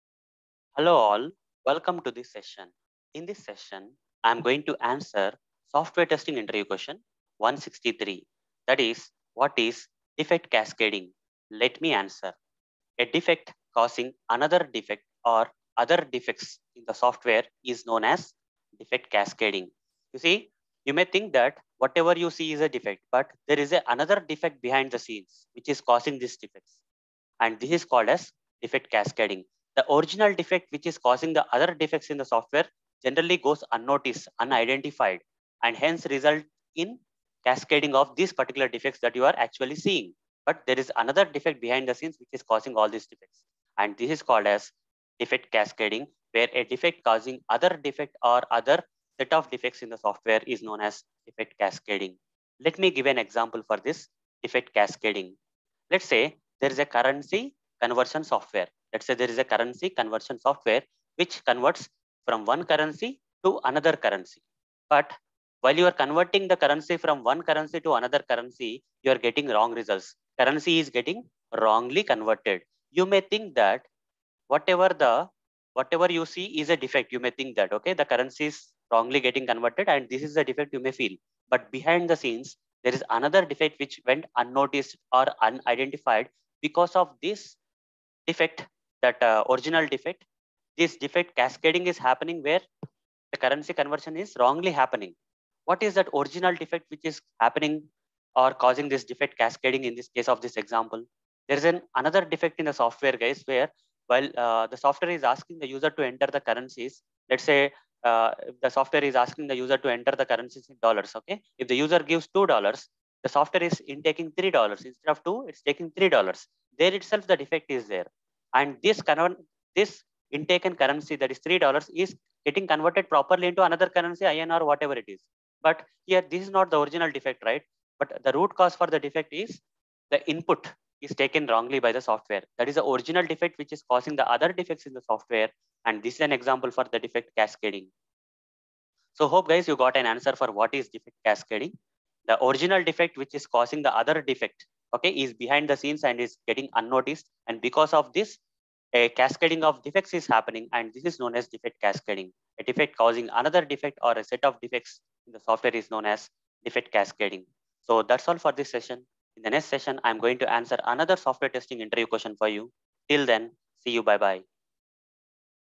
What is Defect Cascading? (Software Testing Interview Question #163)
In this session, I have answered What is Defect Cascading?